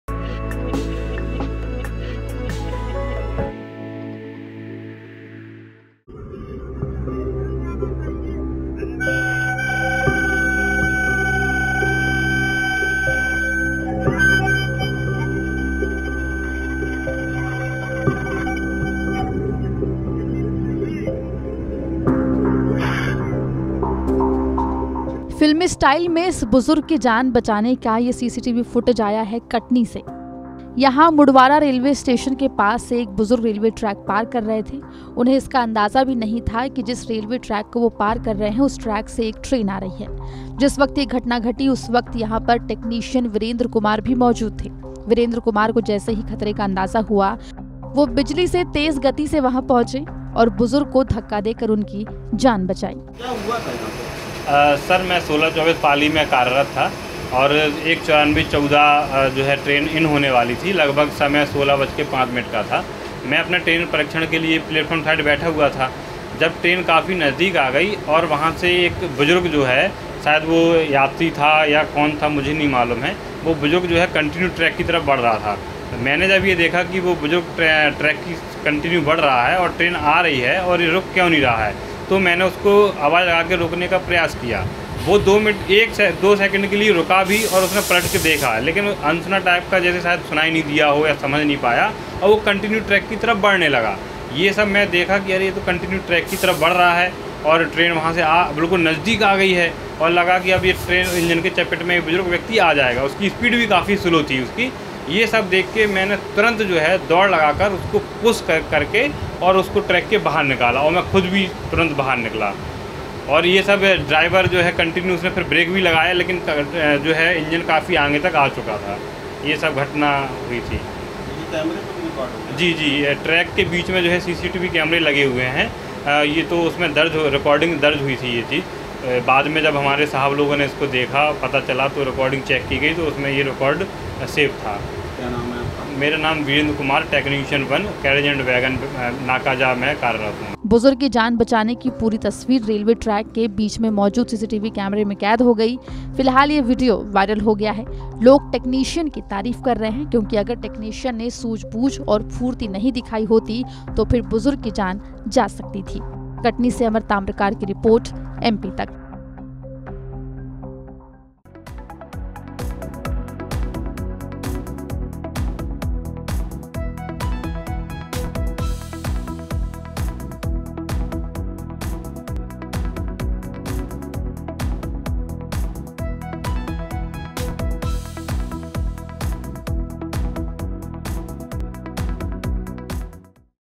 ट्रैक पर आ रही थी ट्रेन और ट्रैक से गुजर रहे बुजुर्ग को बचाने का वीडियो वायरल ! | MP Tak
कटनी के मुड़वारा रेलवे स्टेशन  में केबिन के पास एक बुजुर्ग रेलवे ट्रैक  को पार कर रहा था। तभी अचानक एक ट्रेन बुजुर्ग के बेहद नजदीक आ गई। जिसका वीडियो ट्रैक पर लगे सीसीटीवी में दिखाई दिया।